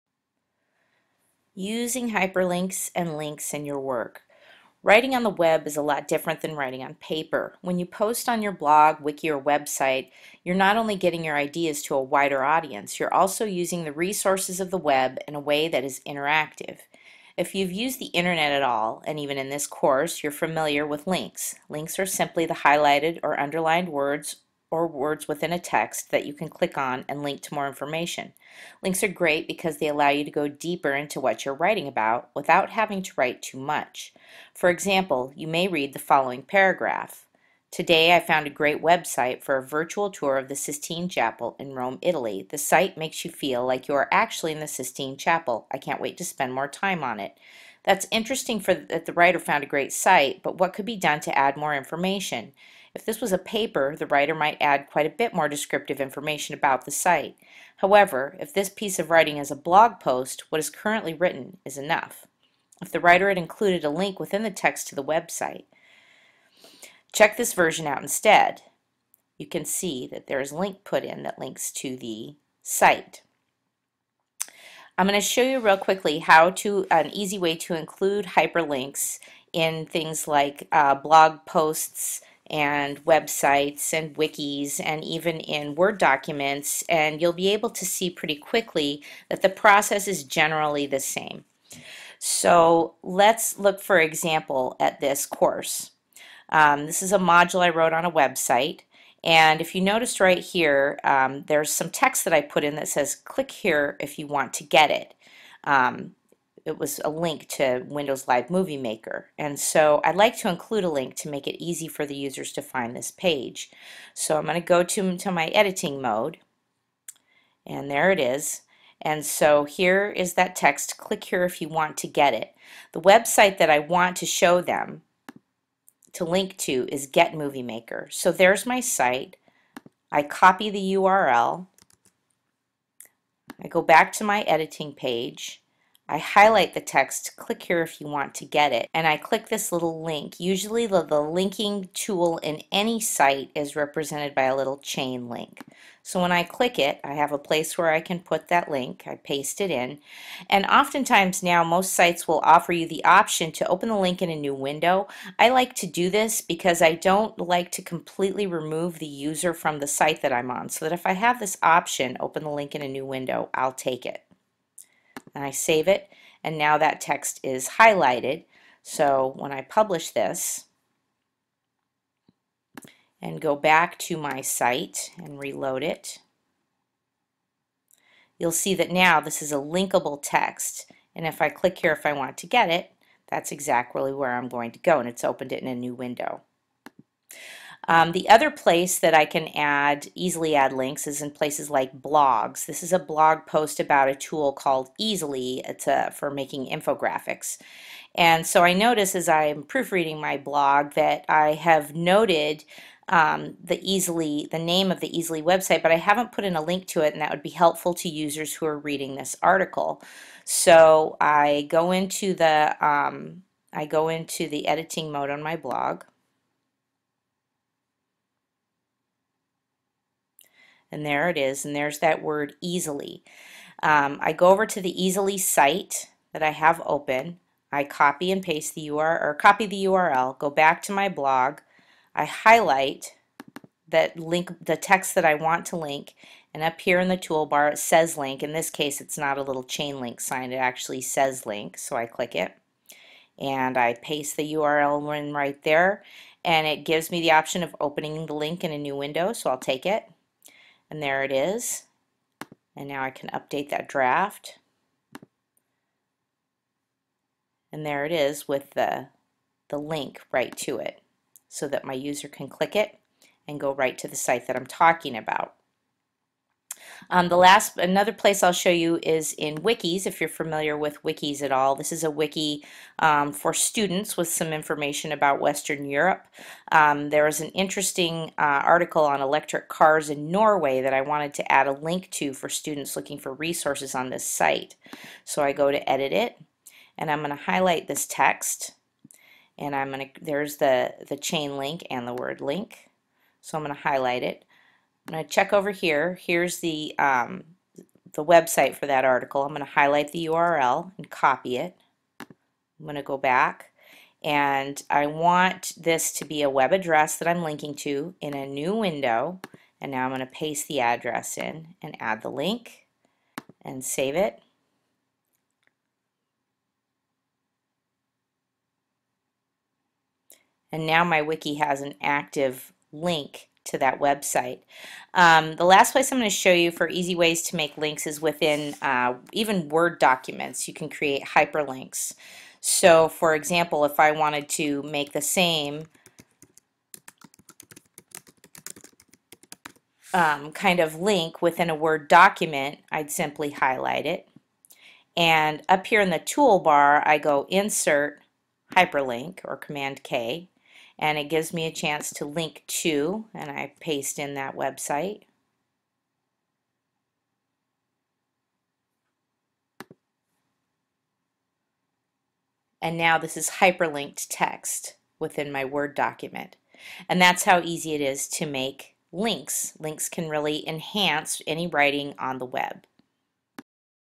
How to make links
How to create links in blogs, websites, wikis, and on documents